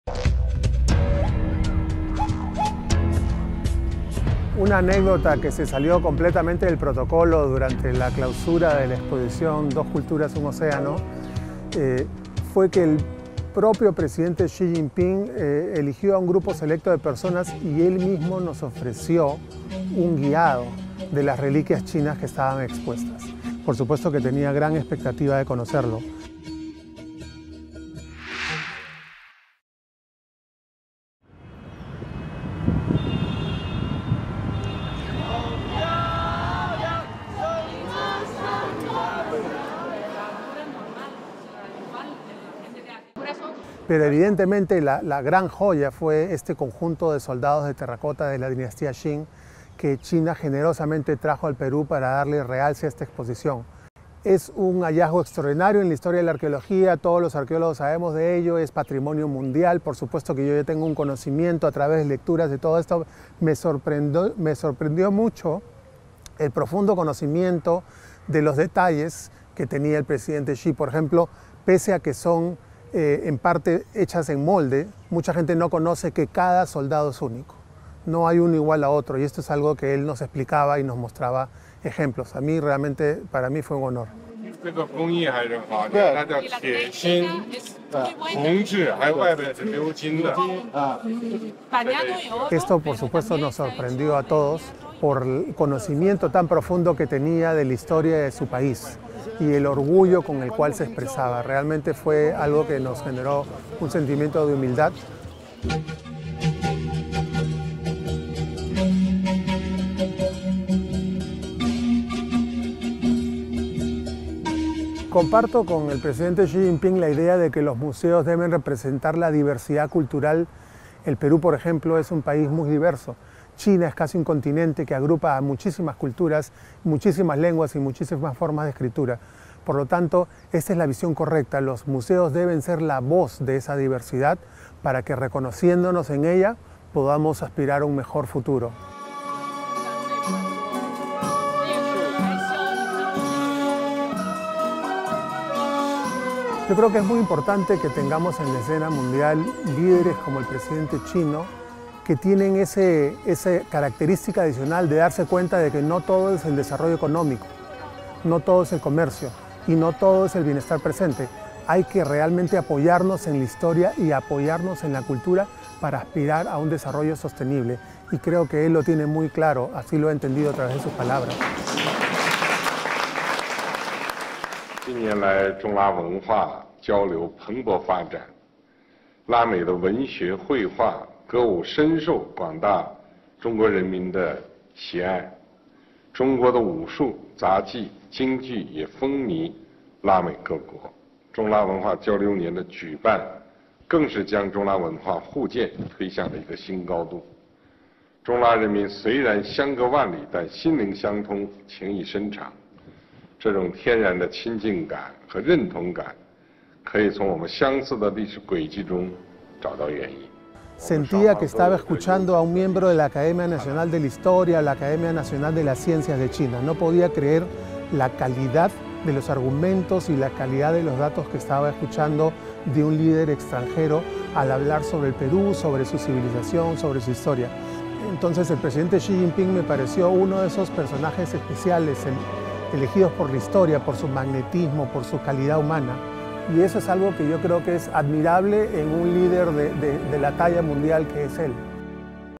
Encounters with Xi Jinping: An advocate of cultural diversity
In 2016, while in Peru, XiJinping visited an exhibition of Chinese cultural artefacts in the capital, Lima. More than simply viewing the display, he made a point of introducing the background of some of the items to a specially-chosen group of people. Among them was museum director Ivan Solis, who concluded that the depth of the Chinese president's knowledge, his charisma and his humanity made him someone who must have been chosen by history.